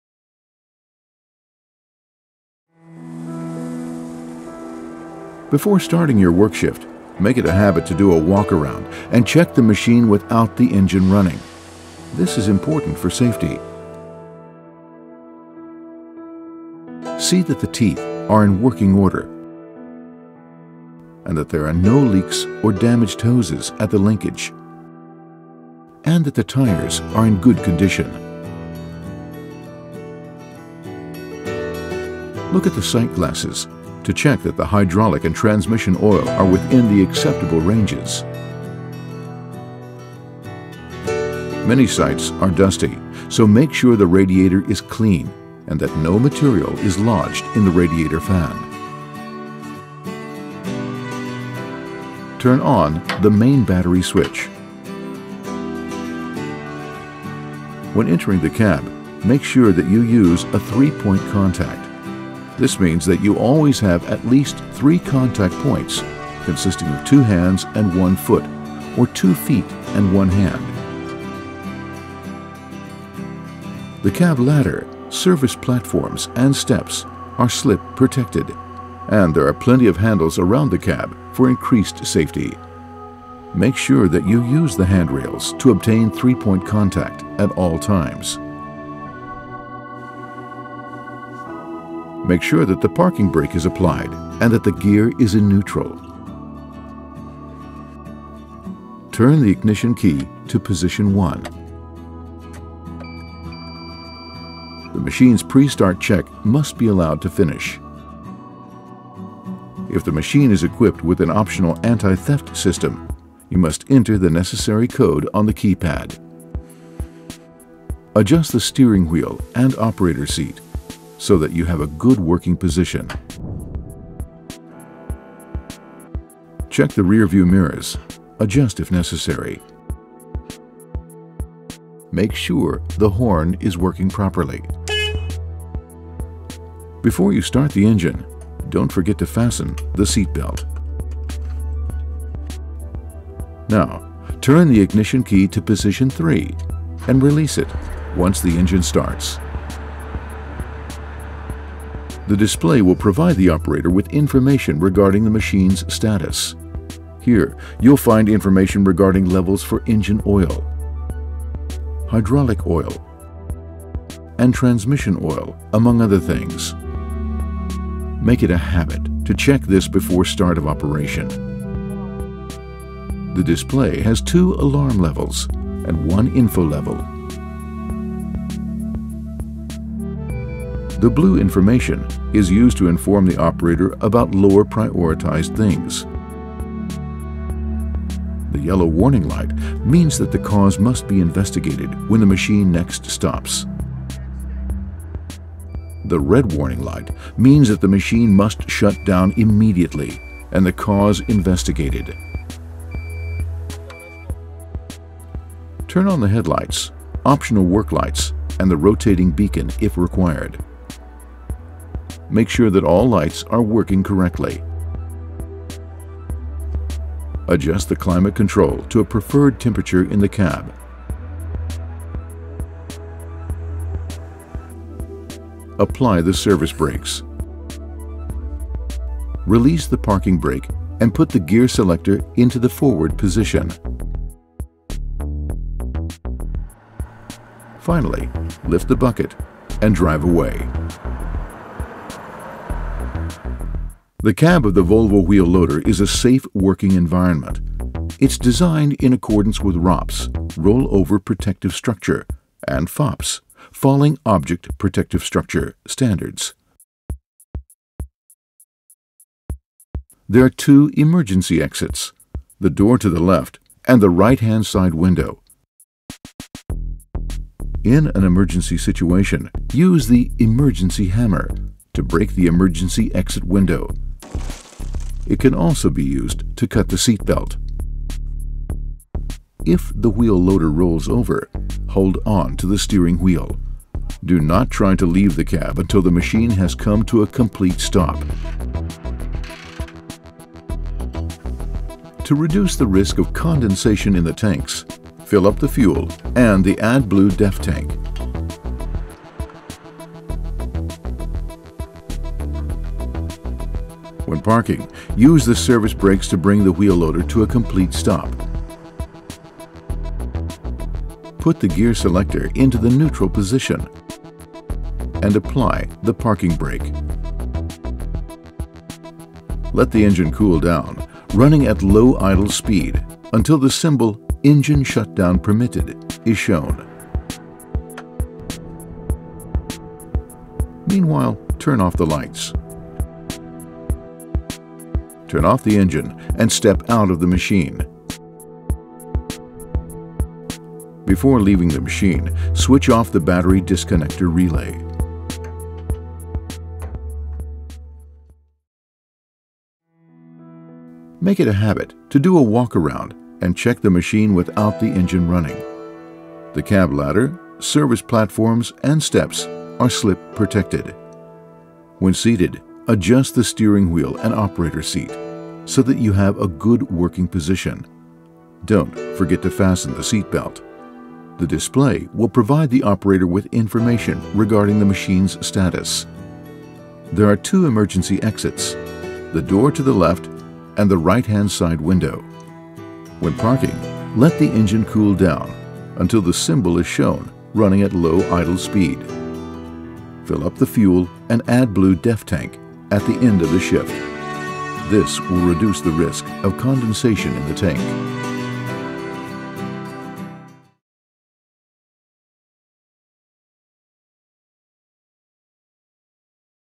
Start and shut down – Volvo Wheel Loaders H-series – Basic operator training – 2/11
Before you start using your Volvo Wheel Loader, you must make sure your machine is working properly. In this video, it is described what you as an operator should do during start and shut down. For example, you should make it a habit to do a walk around and check the machine without the engine running. When parking, let the engine cool down and fill up the fuel and AdBlue/DEF tank at the end of the shift.

Visit the playlist "Volvo Wheel Loaders H-series – Basic operator training" for more instructions.

Please note: This movie is not a substitute for the Operator's manual. IT IS COMPULSORY TO CONSULT THE OPERATOR'S MANUAL FOR COMPLETE DETAILS.

Want to know more about our wheel loaders?